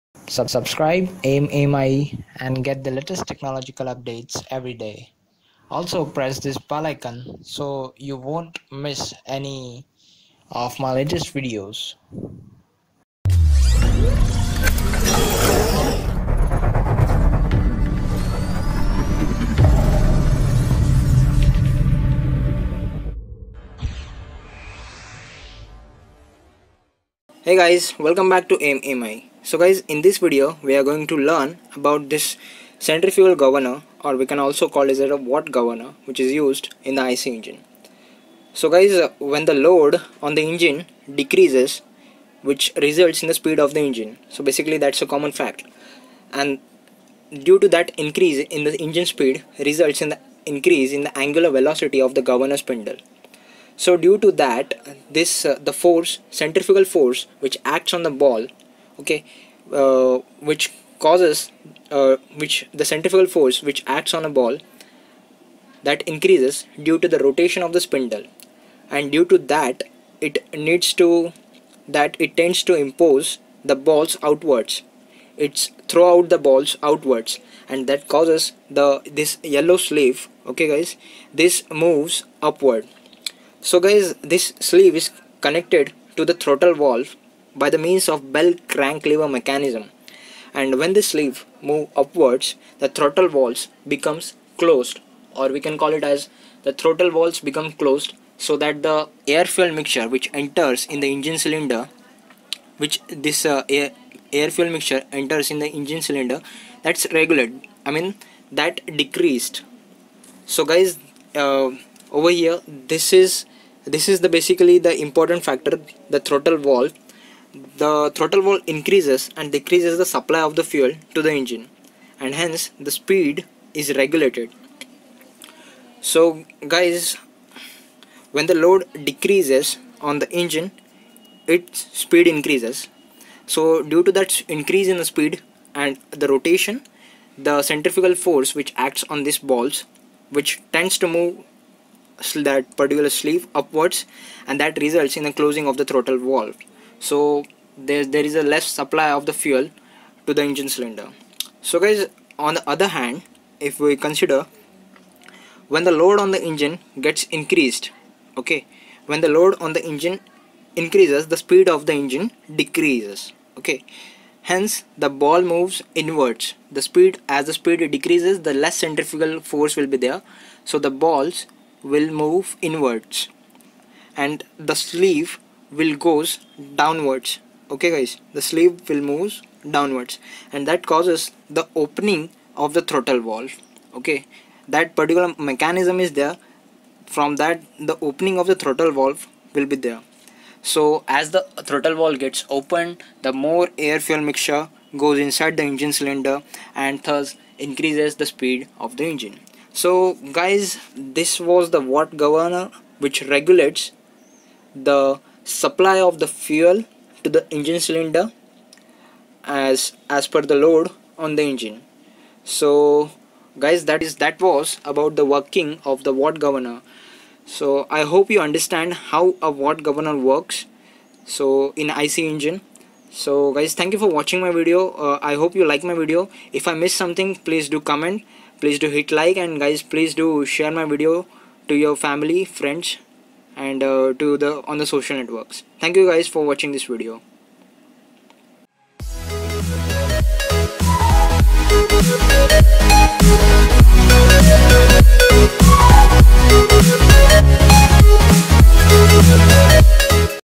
What is Watt Governor?? & how it Works?? ||Engineer's Academy
In this video we will learn the Working of the Watt Governor?? its Working Principle & Why it is Used in IC Engine.

NOTE: All content used is copyright to AIM AMIE